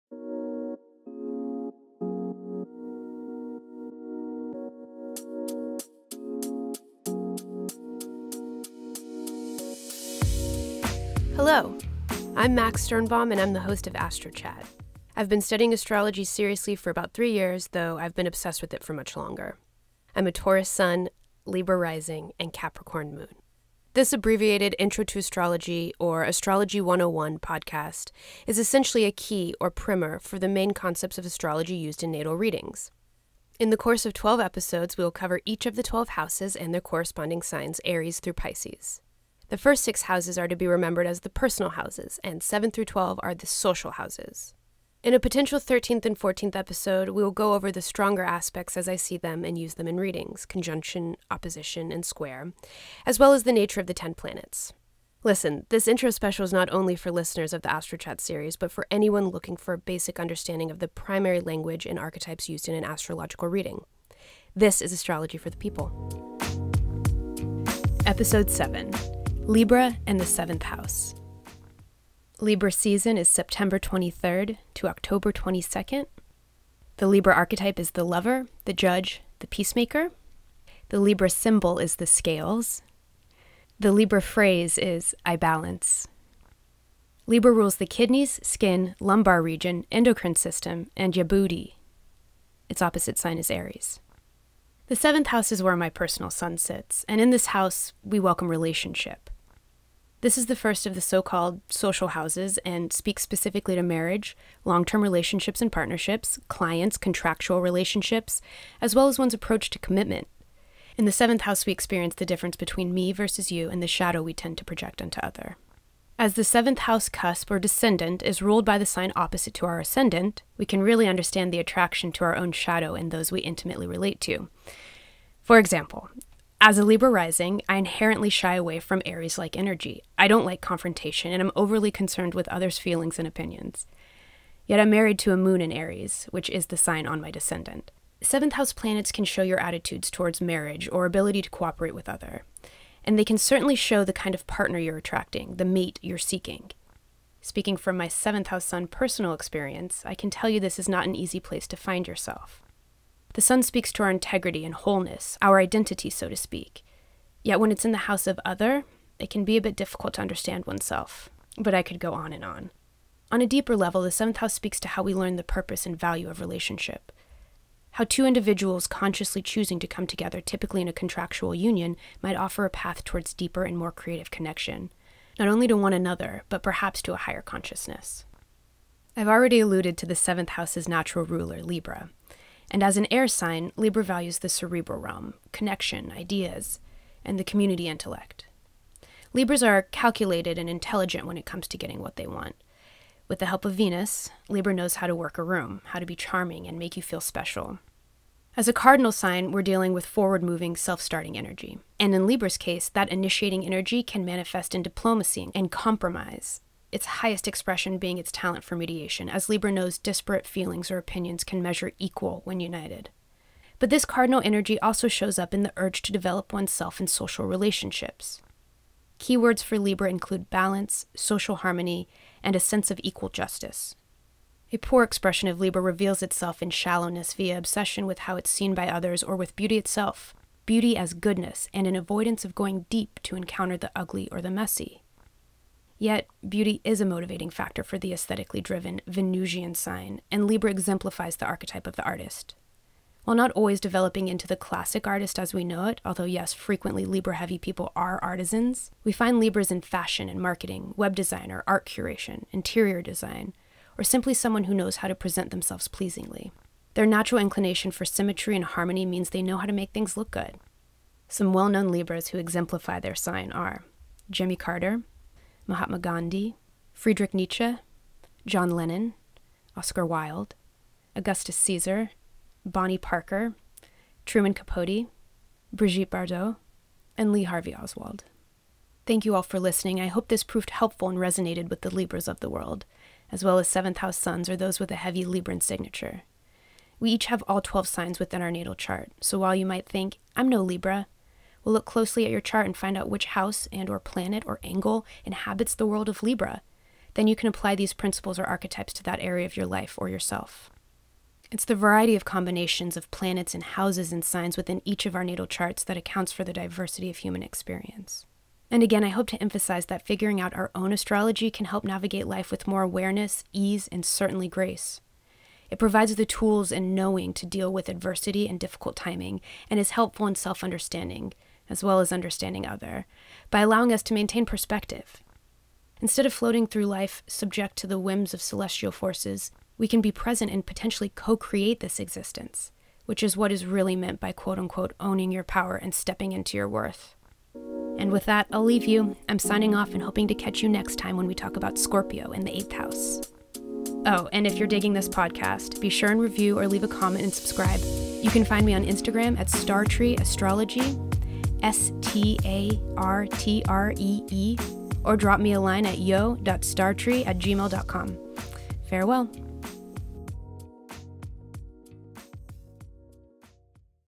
Episode 7: LIBRA LOVE // ASTROCHAT | Snarled Podcast
Et voila, the 7th episode, house, and sign.  Also the first time we encounter *other* in the zodiac. In some ways we can see Virgo's rigorous discipline as preparation for entering into relationship. As a libra rising (and a 7th house sun) I have an affinity for the ability to relate to or level with others that these placements bring. Tag a libra below!

Something Scary
The Shadow Diaries